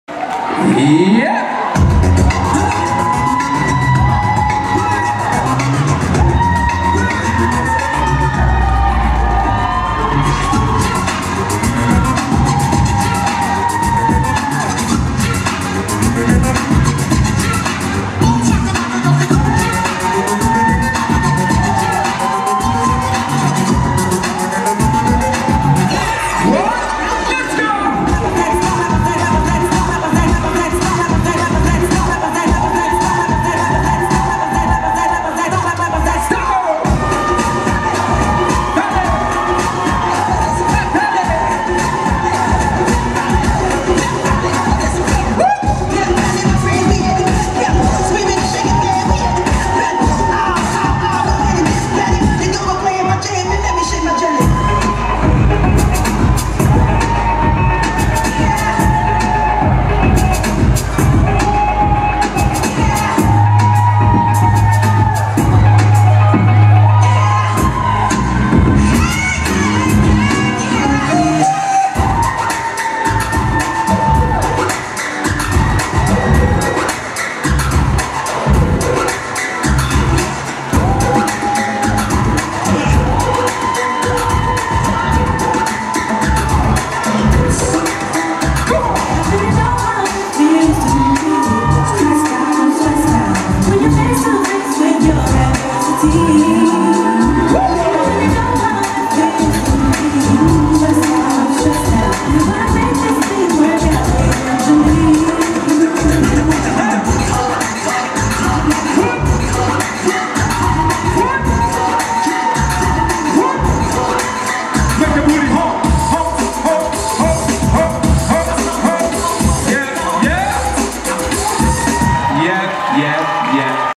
QuerBeatz Seniors 2016
Hier seht ihr unsere Jungsenioren Hip Hop Smallgroup QuerBeatz Seniors bei der TAF Nord-,Nordostdeutschen Hip Hop Meisterschaft 2016.